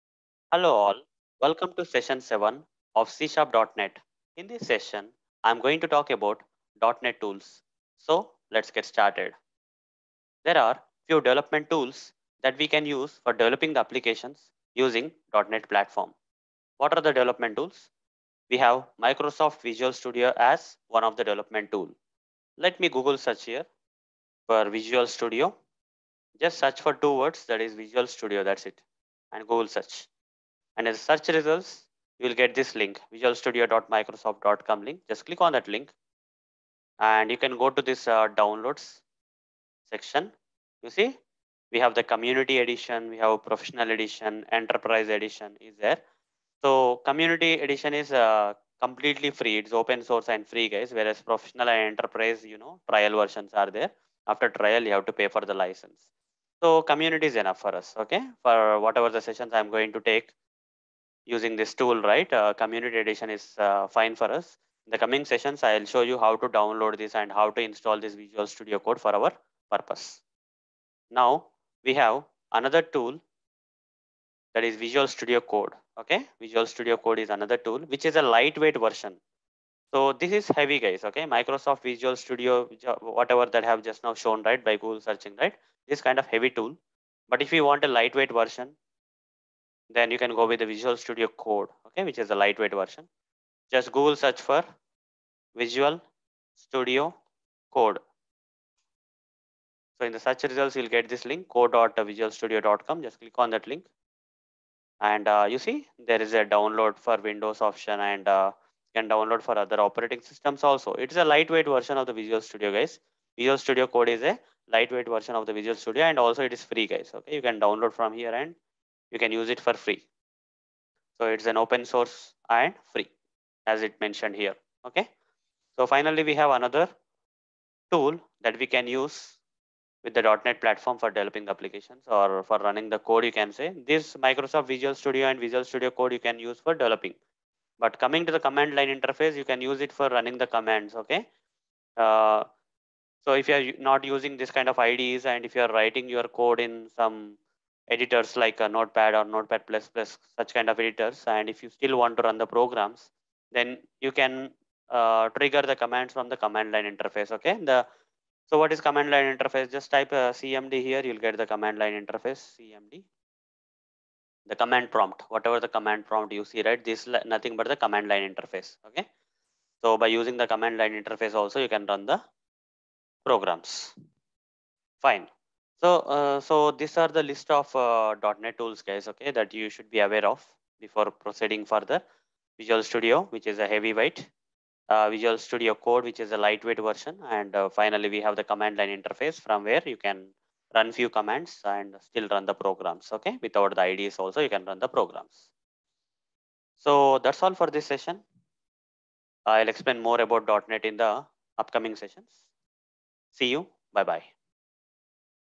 .Net Tools? (C#.Net - Session 7)
In this session, I have explained about .Net Development Tools.